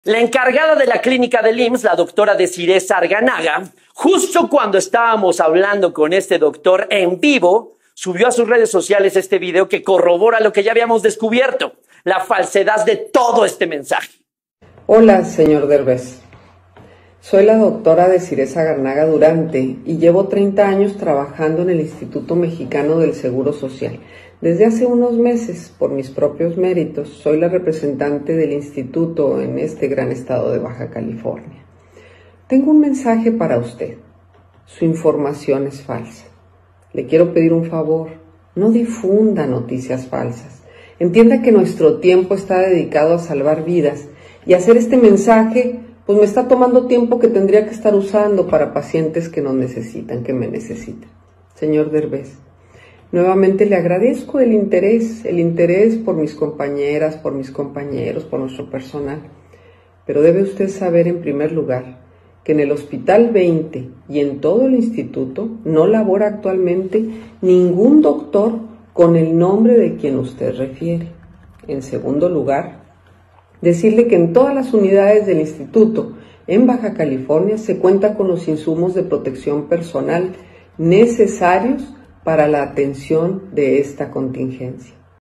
Esto Si Es Feminismo, La Dra Desirée Sagarnaga Desmiente Golpistas
Funcionaria del IMSS exhibe a Eugenio Derbez; “no difunda noticias falsas”
El actor aseguró que un médico amigo suyo, de nombre Faustino Ruvalcaba, le llamó para contarle lo que está ocurriendo en un clínica de Tijuana

Luego de que el actor mexicano, Eugenio Derbez denunciara mediante un video publicado en su cuenta de Twitter que en la clínica 20 del IMSS en Tijuana, Baja California, se vive una situación precaria para pacientes Covid-19 y denunciara la falta de insumos médicos para atenderlos, autoridades del instituto lo desmintieron y exhibieron.

A través de un video, el actor aseguró que un médico amigo suyo, de nombre Faustino Ruvalcaba, le llamó por teléfono para contarle lo que está ocurriendo en dicha clínica. Aseguró que la situación ha sido rebasada ante la gravedad y número de los casos que se han presentado en dicho nosocomio.

Ante esto el IMSS respondió al actor y fue la maestra en Sistemas de Salud, Titular del Órgano de Operación Administrativa Desconcentrada del IMSS en Baja California y San Luis Río Colorado, Sonora, quien mediante un video también rechaza la supuesta denuncia de Derbez.

“Son las 11:15 pm y estoy terminando una jornada de más de 18 horas en @IMSS_BC, pero antes de irme tengo que mandar el siguiente mensaje al señor @EugenioDerbez para hacerle saber que la información que hoy difundió, es falsa. BastaDeFakeNews“, respondió la doctora a Eugenio Derbez.

“Hola señor Derbez, tengo 30 años trabajando el Instituto Mexicano del Seguro Social… Tengo un mensaje para usted su información es falsa, le quiero pedir un favor; no difunda noticias falsas, entienda que nuestro tiempo está dedicado a salvar vidas y hacer este mensaje me está tomando tiempo que podría estar usando para pacientes que me necesitan. Le agradezco el interés por mis compañeros pero debe usted saber que en el hospital 20 y en todo el Instituto no labora actualmente ningún doctor con el nombre de quien usted refiere.

“En segundo lugar decirle que en todas las unidades del Instituto en Baja California se cuenta con insumos de protección personal necesario para la atención de esta contingencia. En tercer lugar lo invito a que si usted no me cree nos visite cuando guste y de verdad con mucho gusto le mostramos los equipos de protección con que el personal trabaja y le presentamos el cuerpo de gobierno del hospital actualmente al frente de la atención a los pacientes”, dijo

La especialista reconoce que han existido problemas, como todo el mundo, pero aseguró que no han parado para resolverlos lo más pronto posible, “pero detener la pandemia de Covid-19 se hace mucho más difícil cuando se propaga la otra pandemia, las fake news"